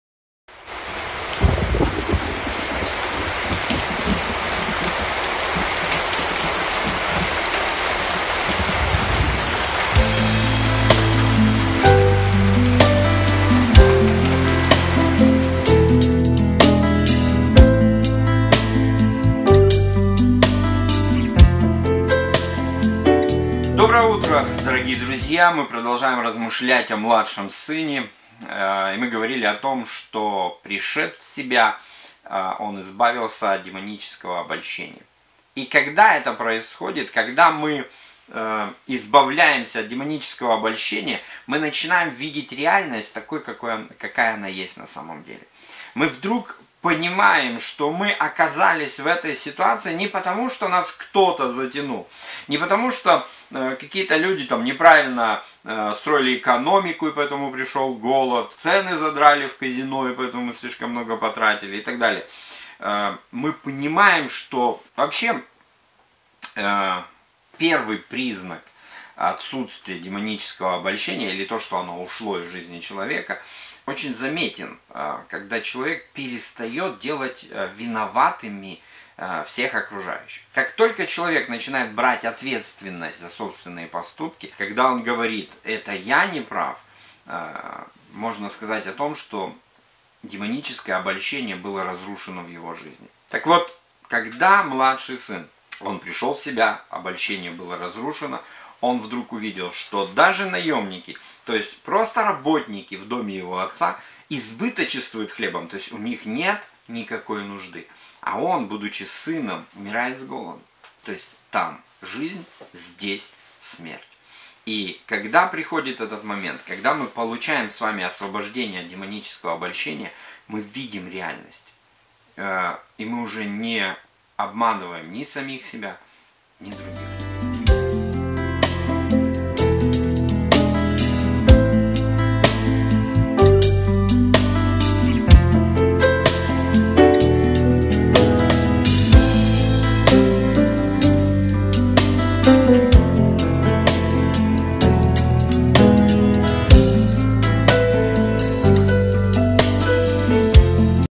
РЕАЛЬНОСТЬ
Освободившись от демонического обольщения, мы начинаем понимать, что виноваты не только обстоятельства и другие люди, но, в первую очередь, мы сами.